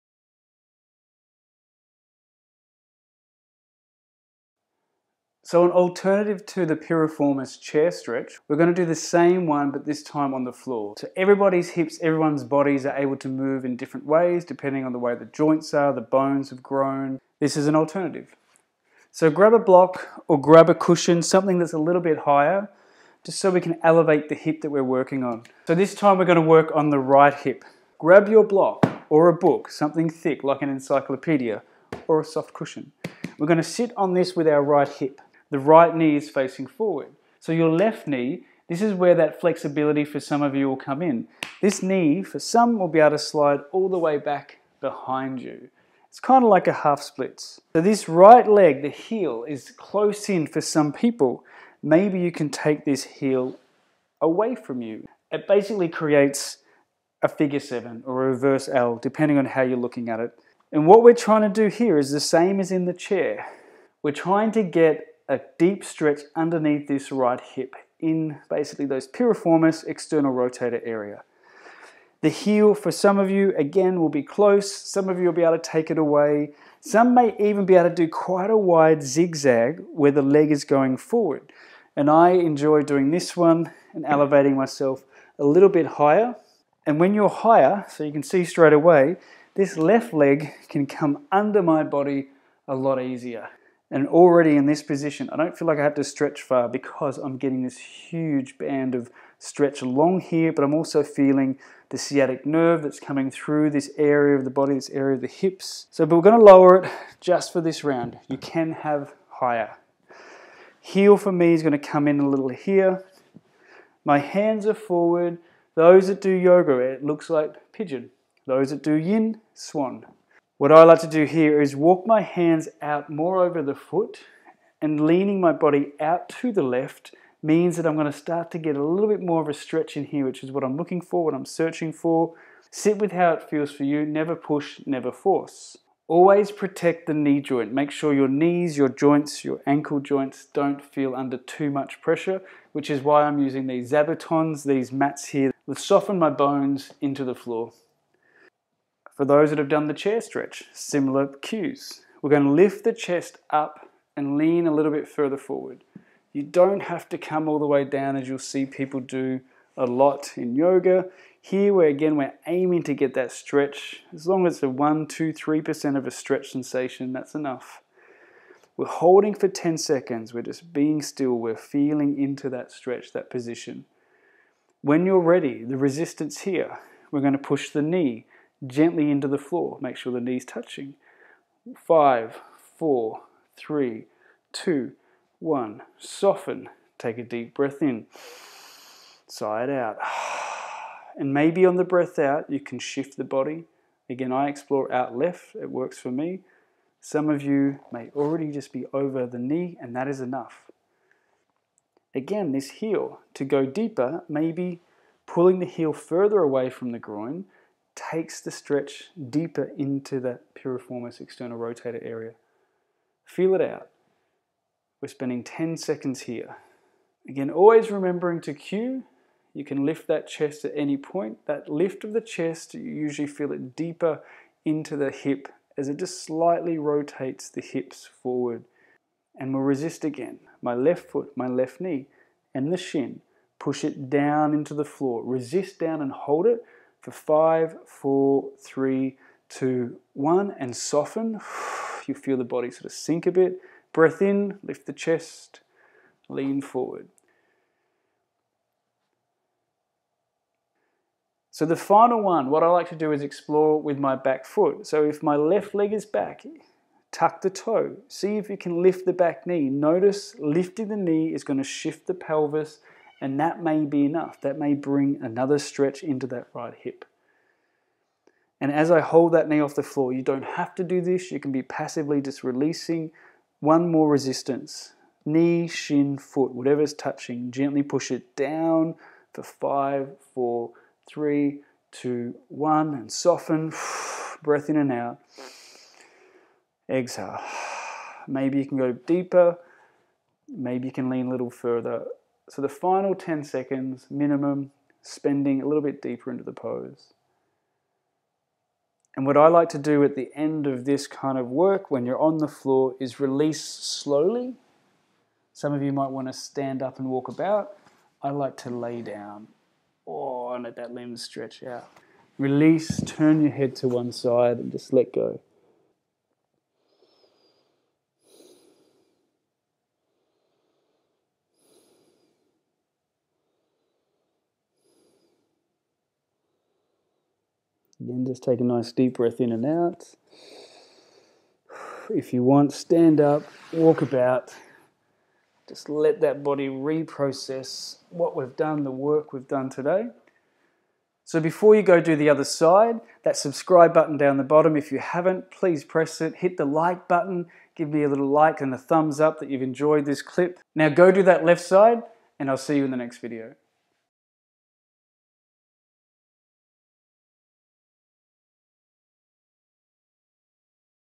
Floor Piriformis Stretch - Sciatica relief
A Stretch Therapy variation of the Piriformis / External rotator PNF stretch.

Piriformis is one of the External Rotator muscles.

The lifestyle of sitting can cause lower back issues & piriformis syndrome which can lead to sciatica nerve pain felt down the leg.

This PNF Stretch Therapy technique can help relieve sciatic pain, strengthen the external rotators & improve the range of motion in the external rotators.  And is a great counter stretch to the life we live where we find ourselves in a seated position for up to 15 hours a day.